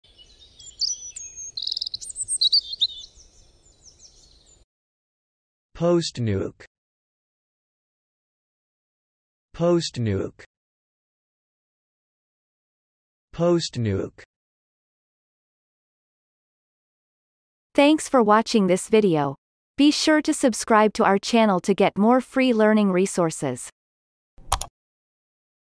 How to pronounce postnuke - Vocab Today
The pronunciation of the word postnuke in Amercian accent is demonstrated in this video. If the word is from another language, such as brand name, it will be pronounced in its original language. Based on the word frequency, this is of 5 stars difficulty.